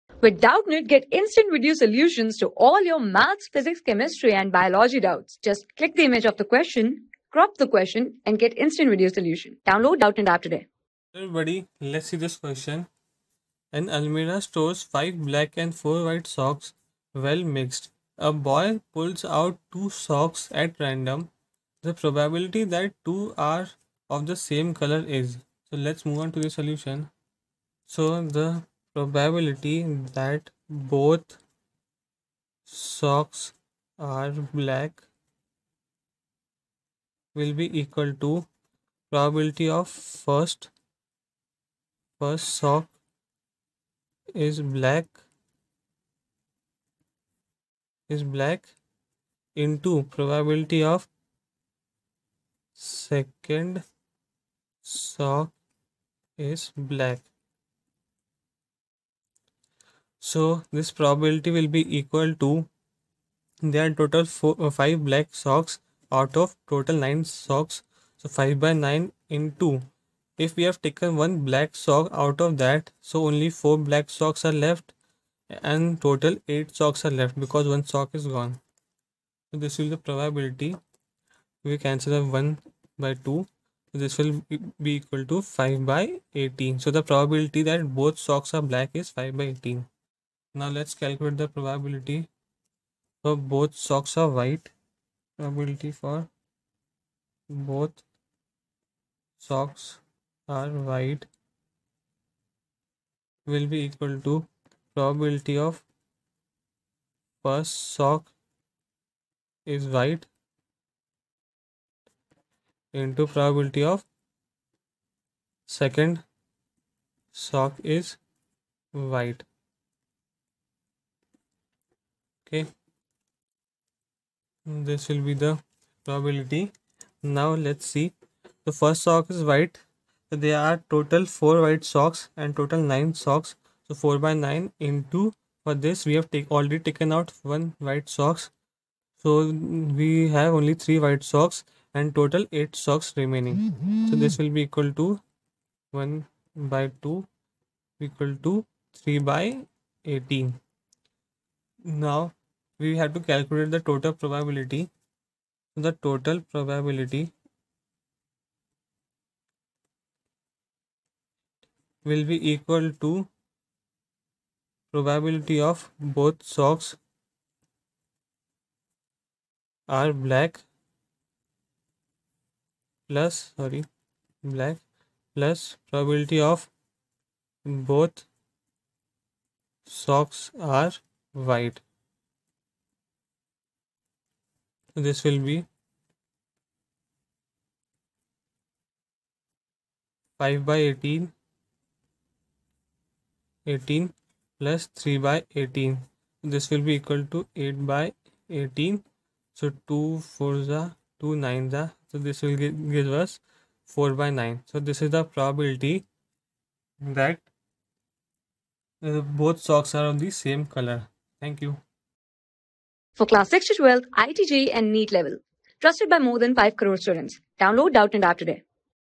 An almirah stores 5 black and 4 white socks well mixed. A boy pulls out 2 socks at random. The p...
An almirah stores 5 black and 4 white socks well mixed. A boy pulls out 2 socks at random. The probability that 2 are of the same colour is
Class: 11
Subject: MATHS
Chapter: PROBABILITY
Board:IIT JEE

👉 Have Any Query? Ask Us.